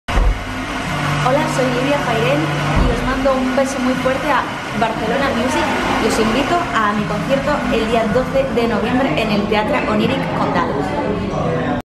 Lydia Fairén nos invita a su primer concierto On my own en Barcelona
"On My Own” es el primer concierto en solitario de la actriz catalana de musicales Lydia Fairén, acompañada al piano y dirigida por Xavier Torras.